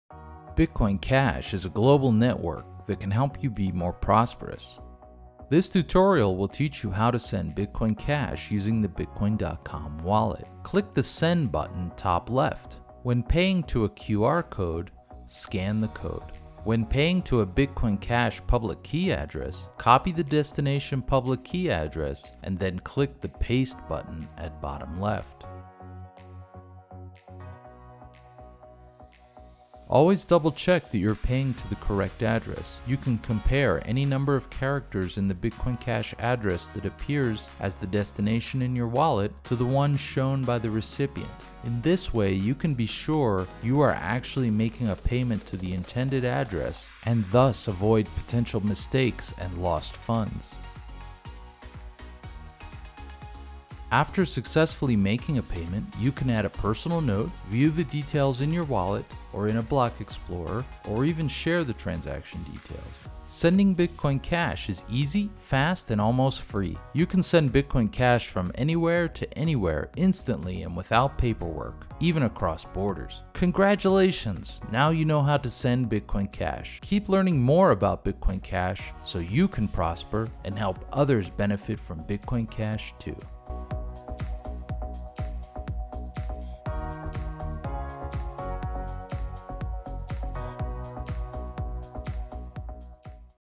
How to Send Bitcoin Cash Using the Bitcoin.com Wallet
Learn how to send Bitcoin Cash anywhere almost for free using the Bitcoin.com wallet.